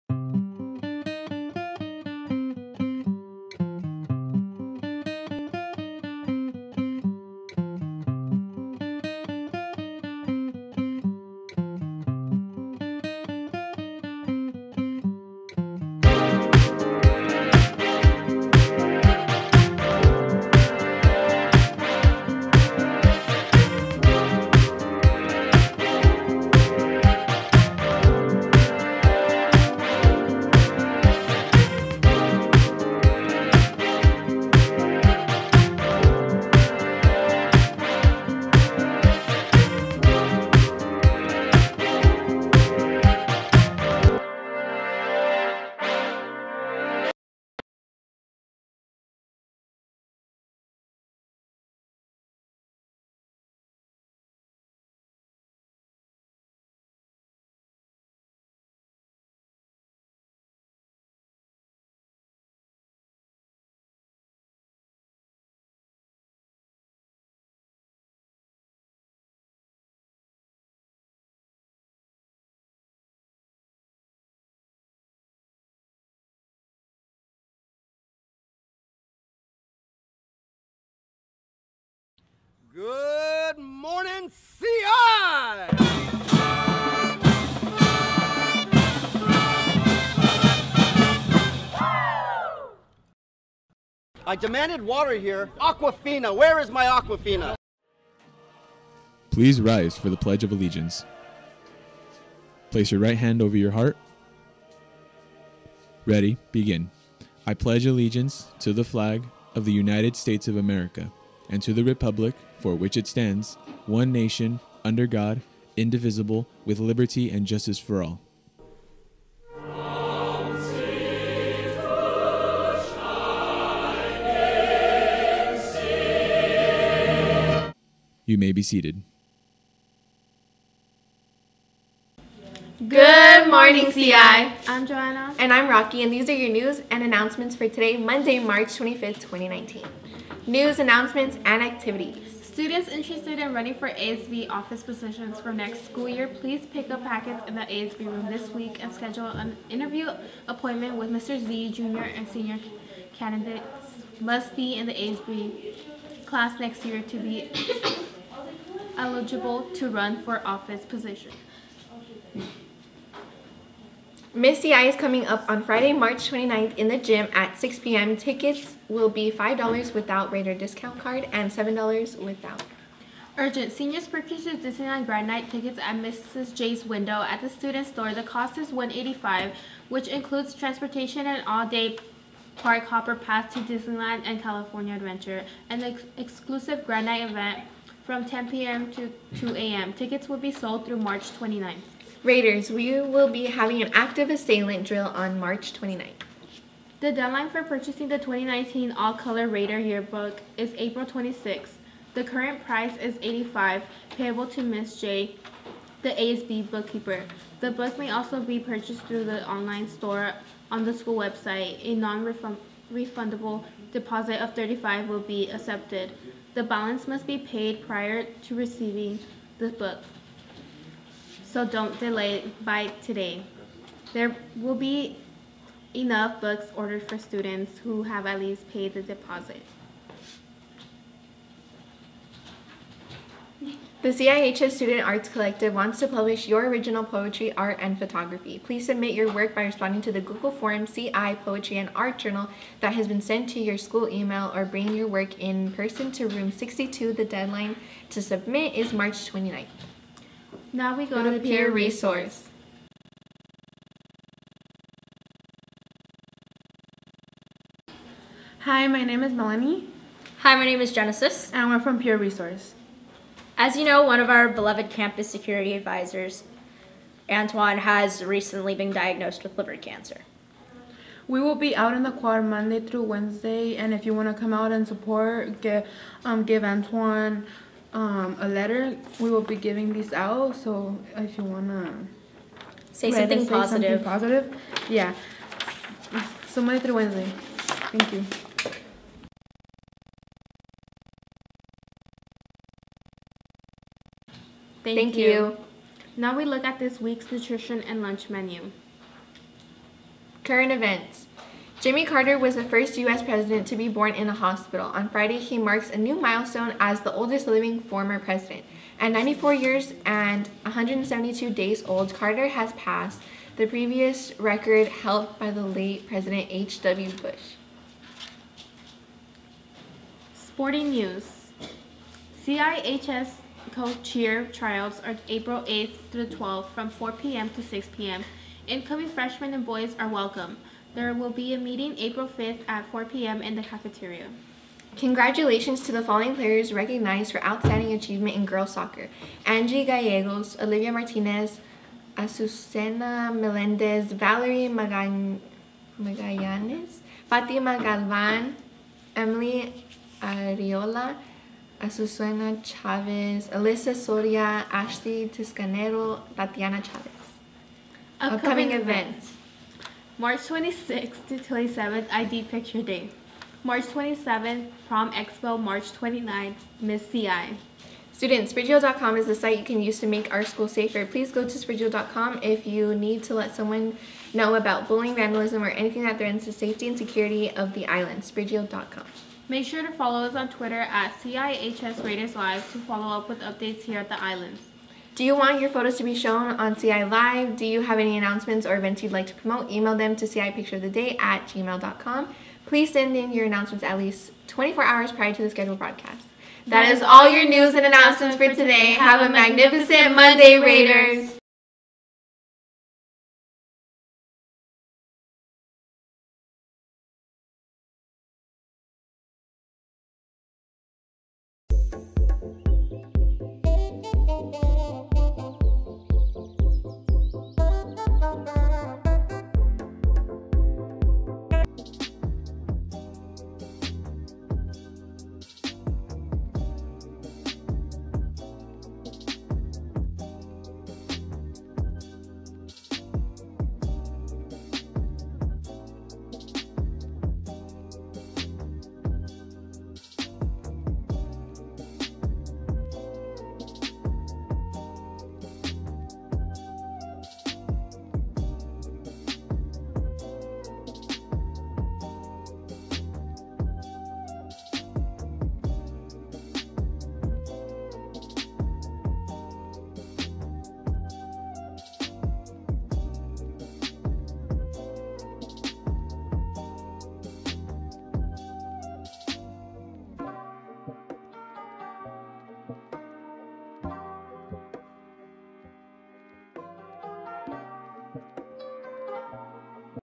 Monday March 25th, 2019 - Peer Resource segment on Antoine L.
It's a sad day at the Islands with the news about our beloved campus supervisor, Antoine. Please teachers have your students watch as Peer Resource shares a powerful message of how you can show him and his family your appreciation. We love you Antoine!